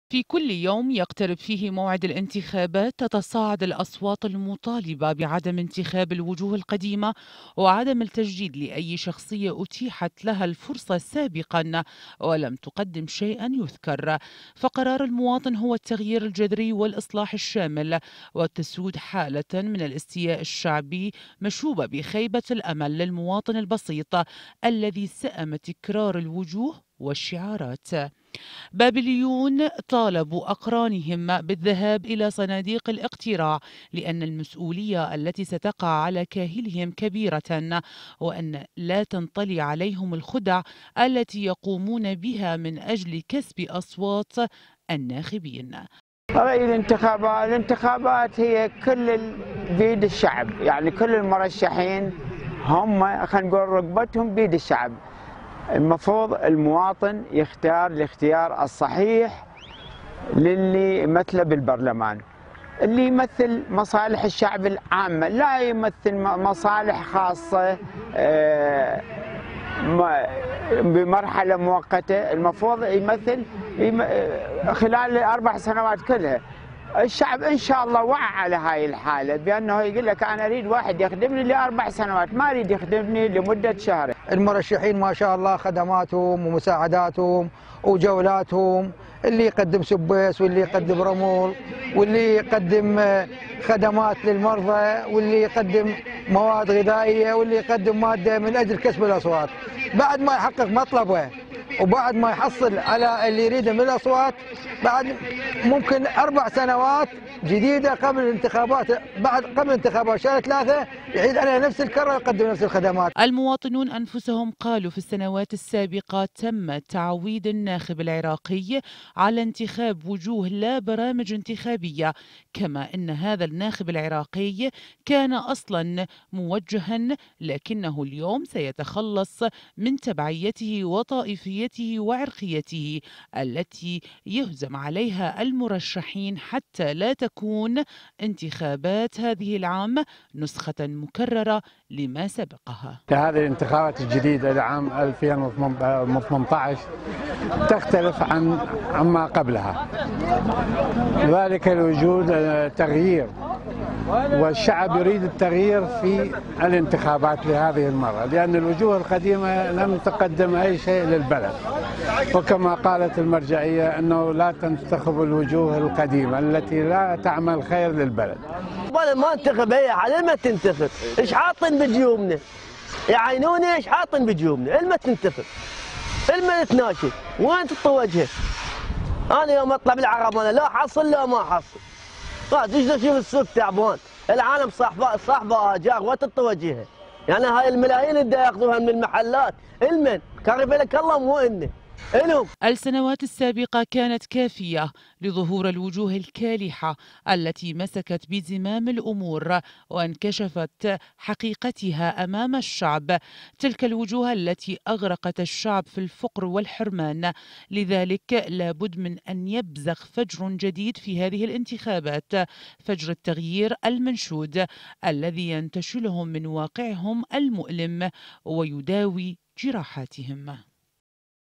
محافظة بابل .. مطالبات بتغيير الوجوه وعدم الوقوع ضحية لمغريات المرشحين
تتعالى اصوات العراقيين في كل مكان مطالبة بالتغيير وعدم انتخاب الوجوه القديمة التي جلبت الدمار للعراق وابنائه.
قراءة: رشا الهيتي